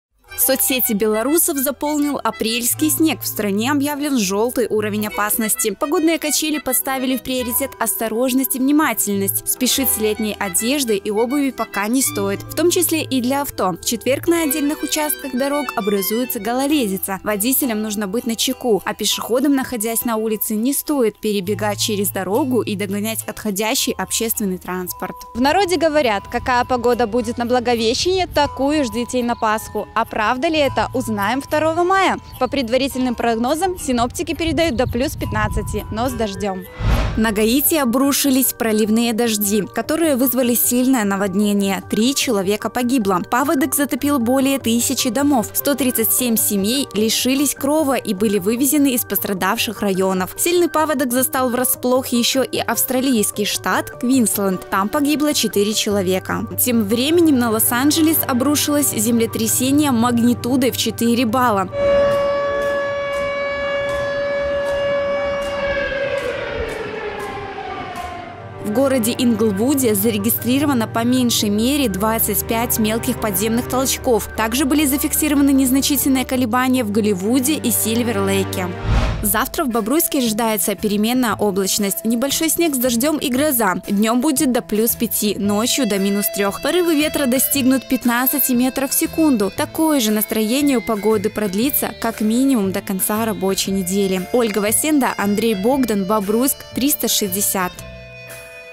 Апрельский снег: что будет дальше?
Апрельские качели. Сегодня в Бобруйске погода вела себя как настоящая женщина – то мокрый снег, то град, то солнце – в общем капризничала.